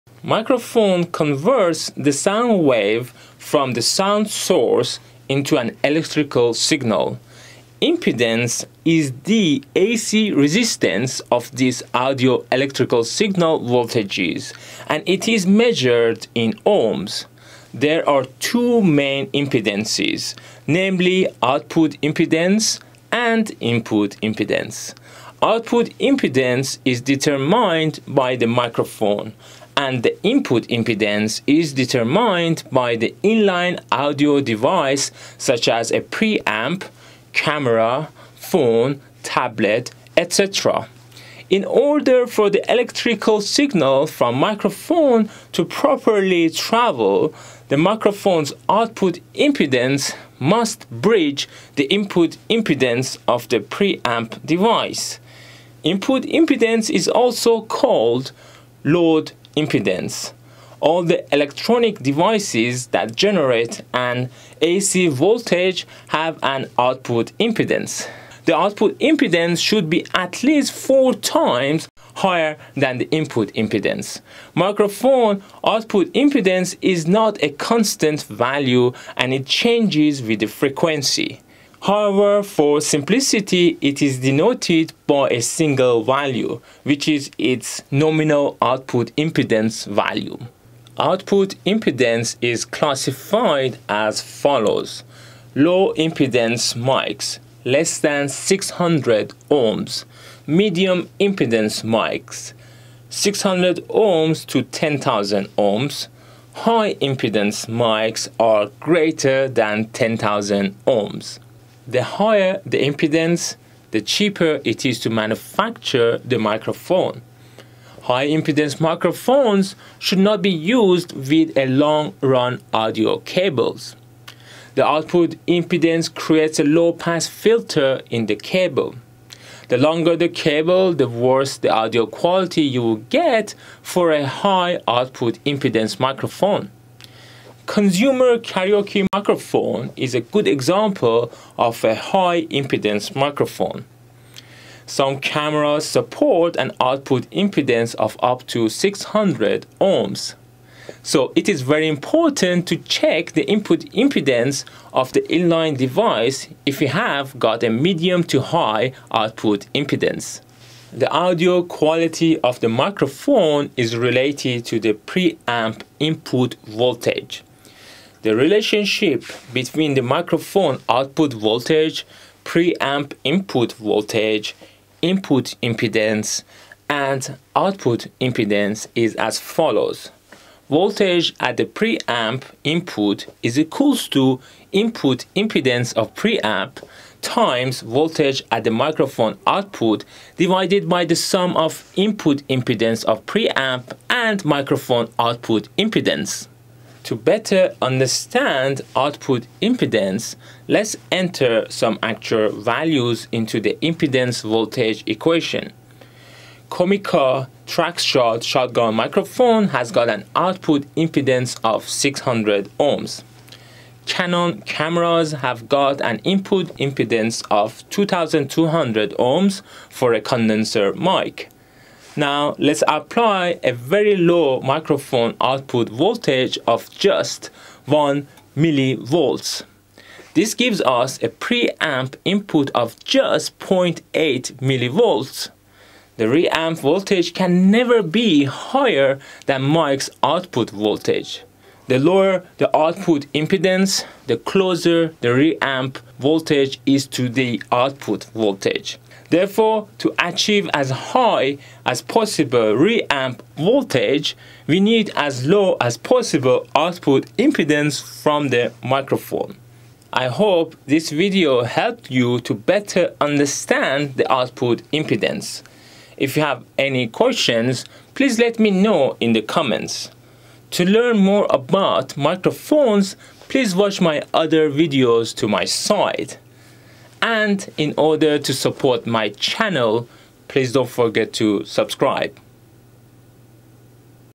What is Microphone Output Impedance
Microphone converts the soundwave from the sound source into an electrical signal. Impedance is the AC resistance of this audio electrical signal voltages and it is measured in ohms. There are 2 main impedances, namely output impedance and input impedance. Output impedance is determined by the microphone and the input impedance is determined by the in-line audio device such as a pre-Amp, camera, phone, tablet, etc.  In order for the electrical signal from microphone to properly travel, the microphone's output impedance must bridge the input impedance of the preAmp device. Input impedance is also called load impedance. All the electronic devices that generate an AC voltage have an output impedance. The output impedance should be at least 4 times higher than the input impedance. Microphone out impedance is not a constant value and it changes with the frequency. However, for simplicity, it is denoted by a single value, which is its nominal output impedance value.

Output impedance is classified as follows
Low-impedance mics: less than 600 Ω
Medium-impedance mics: 600 Ω – 10,000 Ω
High-impedance mics: greater than 10,000 Ω

The higher the impedance, the cheaper it is to manufacture the microphone. High impedance microphones should not be used with a long run audio cables. The output impedance creates a low pass filter in the cable. The longer the cable the worst the audio quality you'll get for a high out impedance microphone. Consumer Karaoke microphone is an example of high-impedance microphone. Some cameras' support an output impedance of up to 600 ohms. So, it is important to check the input impedance of the inline device if you have got a medium to high output impedance.

The audio quality of the microphone is related to the PreAmp input voltage. The relationship between microphone output voltage, PreAmp input voltage, Input Impedance and output impedance is as follows:
Voltage at the pre -Amp input  = Input impedance of pre-Amp x Voltage at the microphone output divided by the sum of Input impedance of pre-Amp & Microphone output impedance)

To understand output impedance better, let's enter some actual values into the impedance voltage equation. Comica Traxshot shotgun mic has got an output impedance of 600 ohm. Canon Camera's have an input impedance of 2200 ohm for a condenser mic. Now, let's apply a very low microphone output voltage of 1 mVolts. This give us a preamp input of just 0.8 mV. The reamp voltage can never be higher than the mic's output voltage. The lower the output impedance, the closer the reamp voltage is to the output voltage. Therefore, to achieve as high as possible reAmp voltage, we need as low as possible output impedance from the microphone